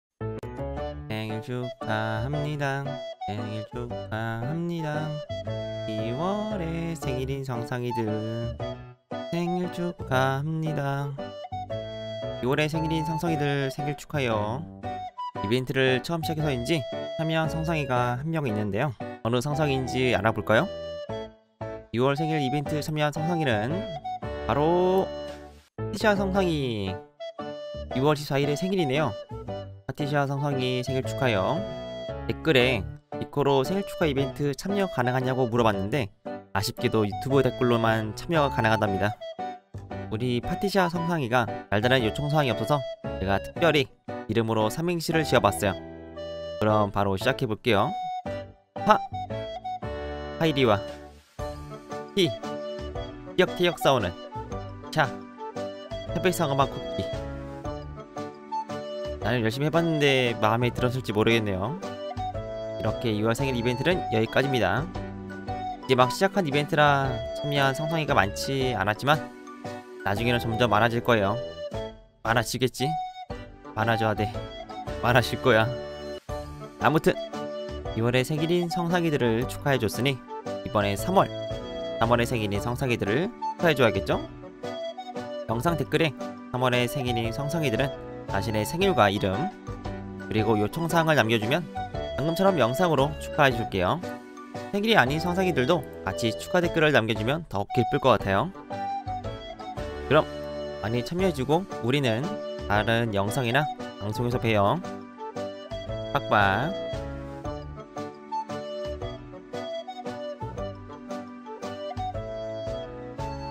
2월에 생일인 솽솽이들 생일 축하해요 ❤ + 3월 생일자 모집! (생일 축하 이벤트)
*참여 방법
[생일 축하 영상]에 댓글로 자신의 이름이나 닉네임을 쓰고 생일과 함께 요구사항도 써주면 됩니다!

*참여 기간
3월 말까지!

*참여 대상
솽솽이 모두!